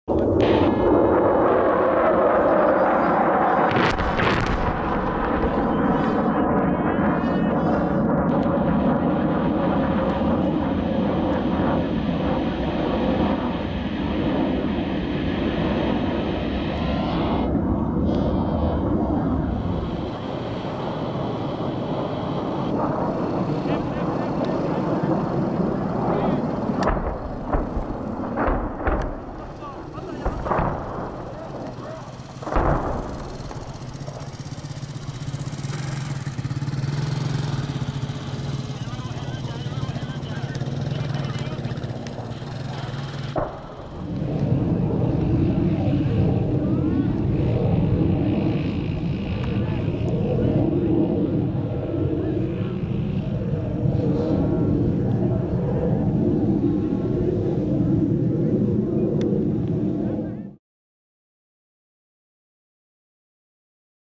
لحظة قصف الطيران الحربي والمروحي قرية التح جنوب ادلب
شنت طيران النظام عدة غارات جوية بالبراميل المتفجرة والصواريخ الفراغية قرية التح بالريف الجنوبي، خلف قتلى وجرحى من المدنيين .